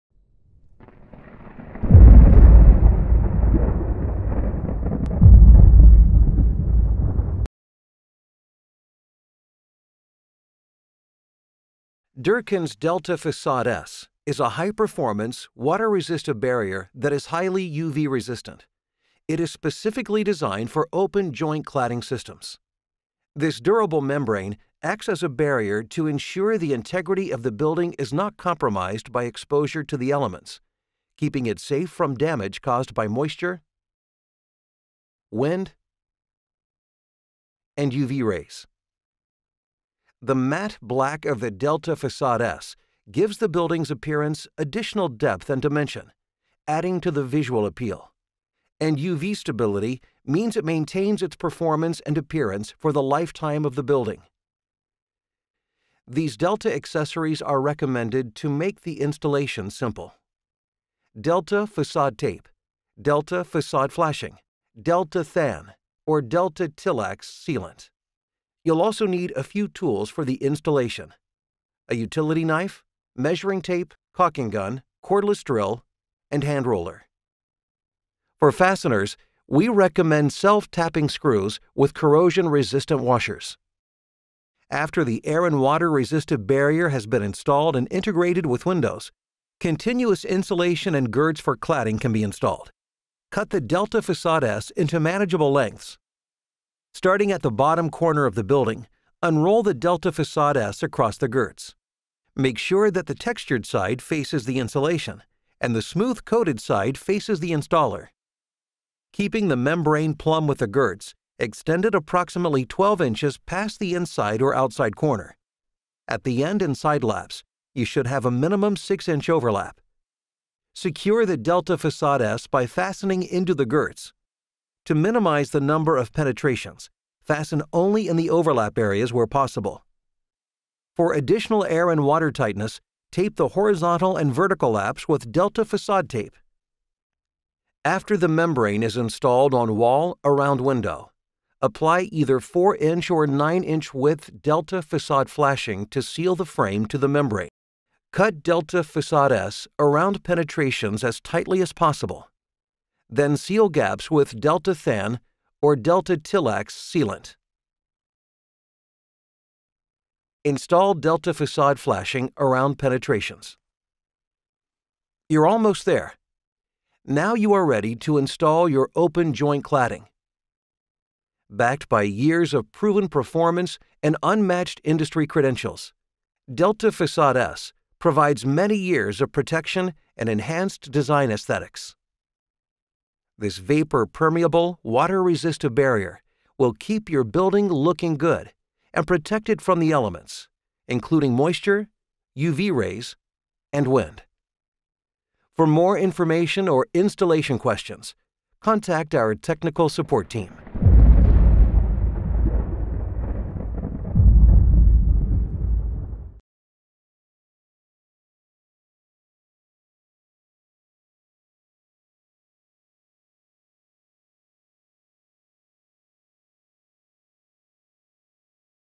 How to install DELTA®-FASSADE S in open-joint cladding designs
The first mechanically fastened, water-resistive barrier designed to maximize building protection without compromising design aesthetics in open-joint cladding applications.

Finalizing your open-joint cladding design is only half the battle; you also need to make sure whatever you’re building stays protected. DELTA®-FASSADE S provides water, wind, and UV protection for your open-joint cladding designs with a water-resistive barrier that lets you build with complete confidence.